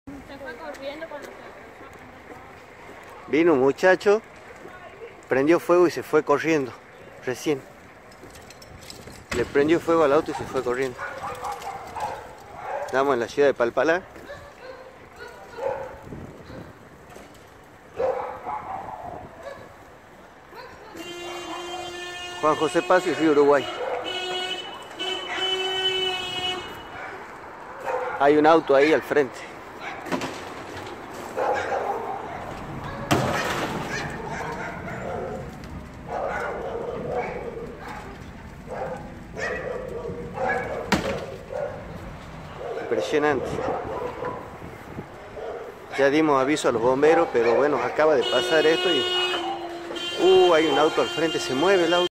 Inseguridad: Incendiaron un auto en Palpalá
La ciudad siderúrgica fue escenario una vez más de un hecho delictivo. En la madrugada del sábado  un sujeto prendió fuego a un Peugeot 504 blanco que se encontraba estacionado en el Bº San Ignacio de Loyola.